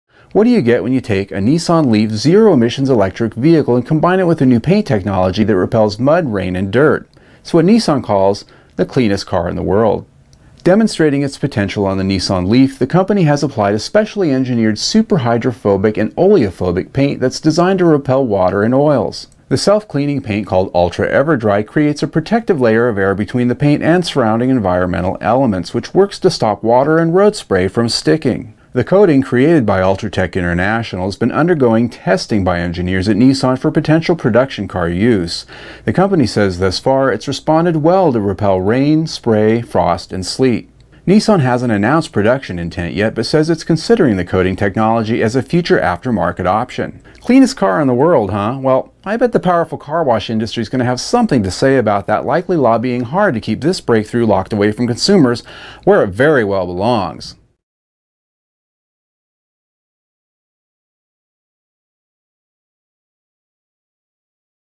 Motoring Minute: Nissan Leaf Nano Paint
Nissan shows off the cleanest car in the world, a zero-emissions electric Leaf with special paint that repels mud, rain and dirt. Nano paint technology demonstrated for future uses.